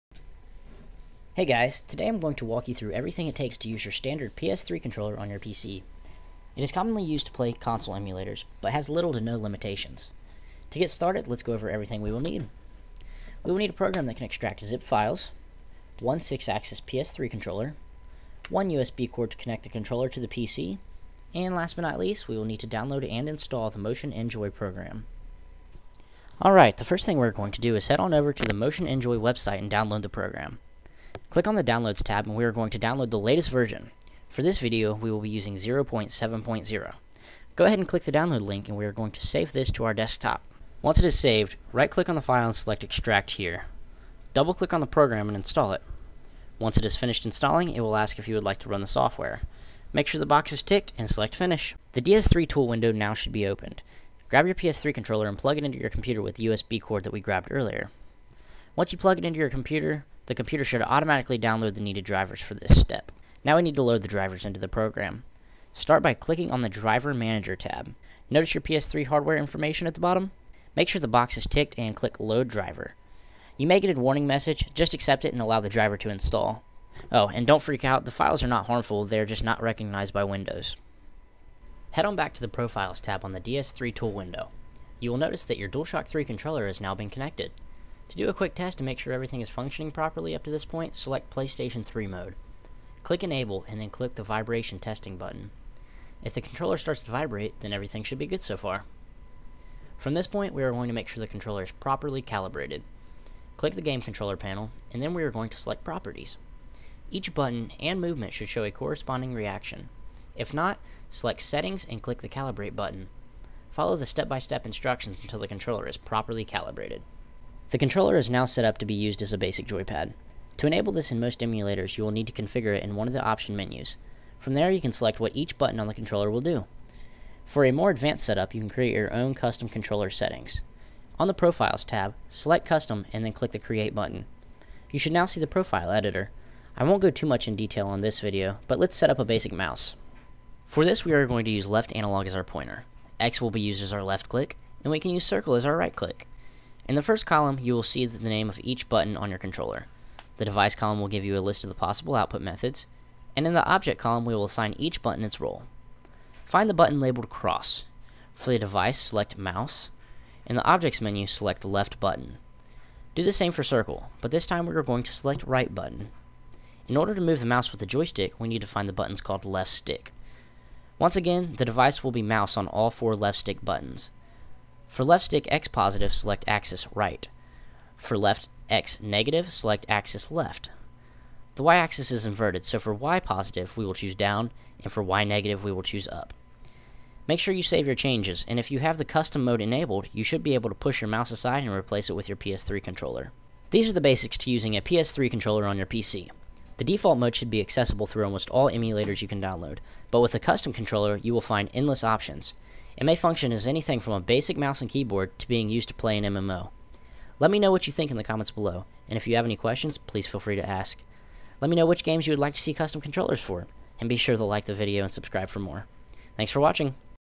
PS3 Controller to PC - How To!
How to use your PS3 controller on your PC.

This tutorial shows you how to download and connect your PS3 controller to your computer.  You can use it for just about anything!